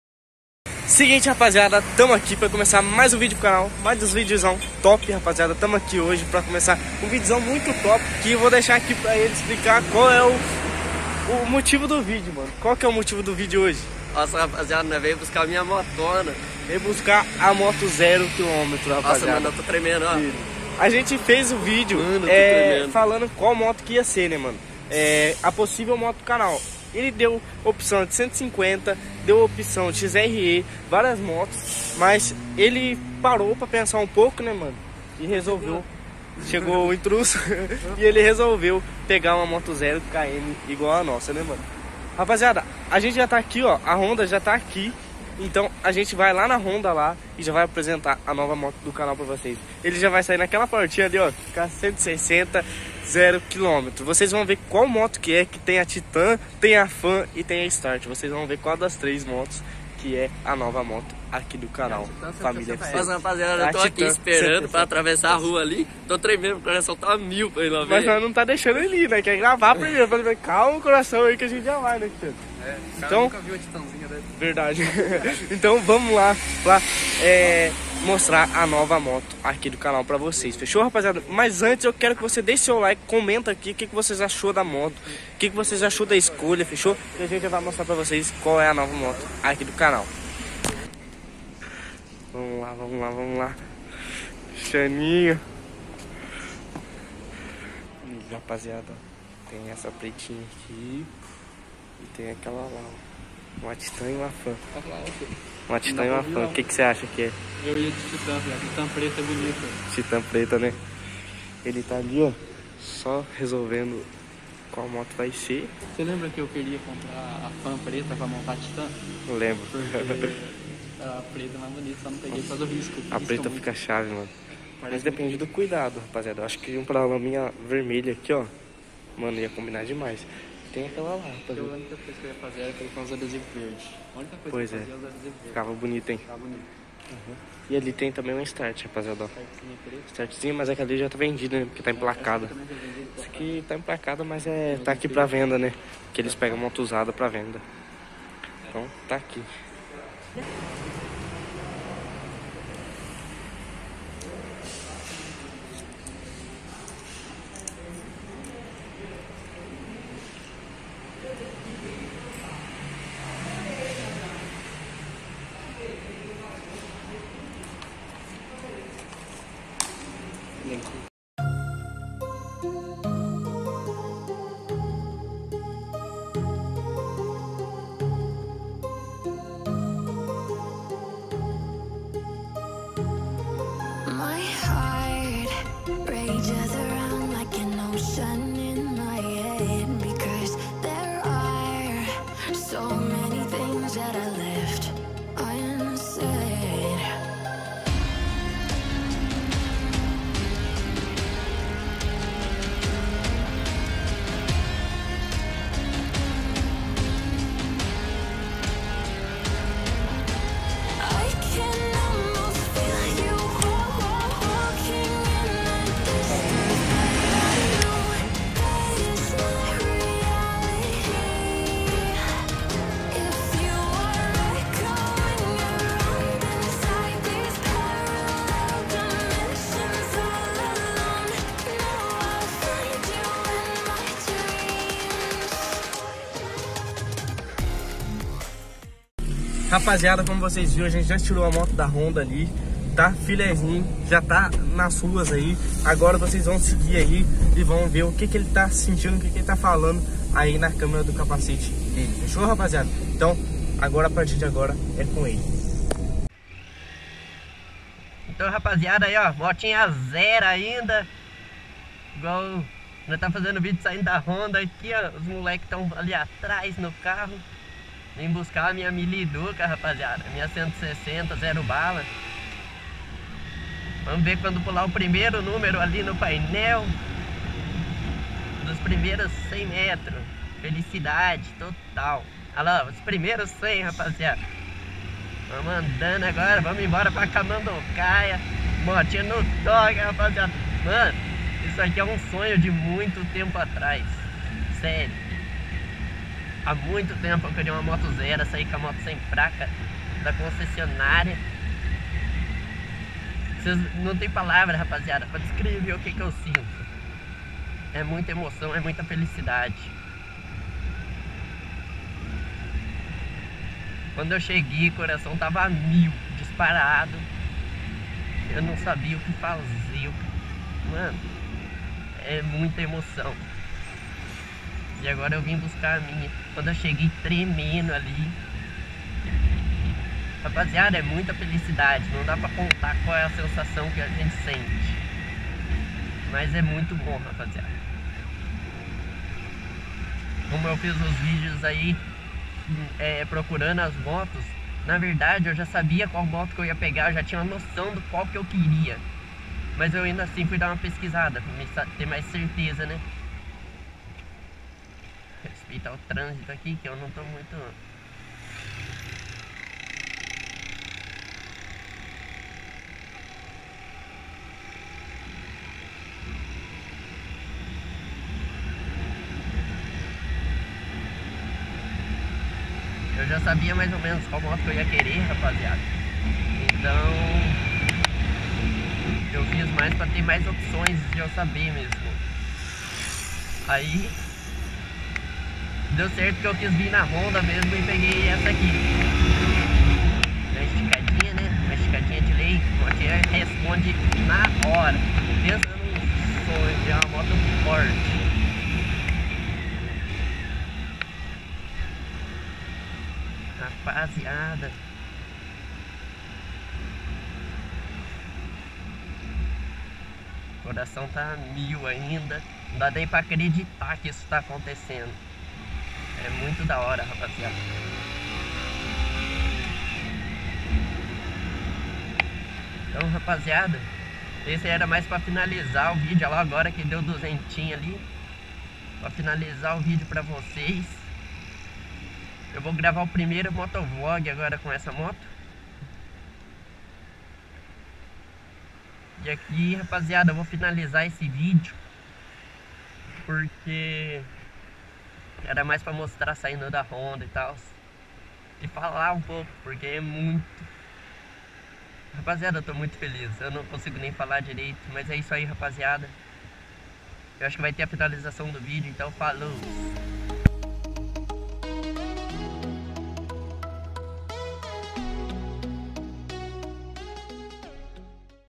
✅[FAN 160 2021] Saindo Com A FAN 160 0km da Honda *Sonho realizado*!
✅[FAN 160 2021] A nova moto do Canal

Salve Rapaziada!
Finalmente chegou o dia!
Fomos buscar a tão sonhada 0km na Honda
Estamos muito felizes com essa nova conquista que será a nova moto do canal, uma FAN 160 2021 prata 0km

✅[PASSO A PASSO] Como colocar as ASAS da TITAN 160 na FAN 160 (Todos os anos)